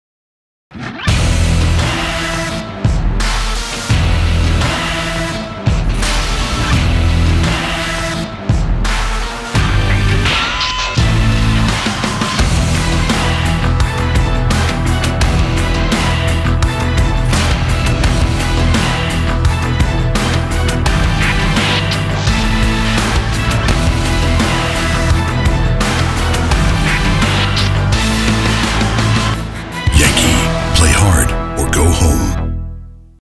Yenkee Gaming mouse HORNET
More powerful, more accurate, more variable, in short, better in every way. This is Hornet AIM.
OMRON switches with a lifetime of 10 million presses
5000 DPI
RGB LED
Pixart 3325
Software
10 fully programmable buttons
Variable weight
Braided cable
Gold-plated USB connector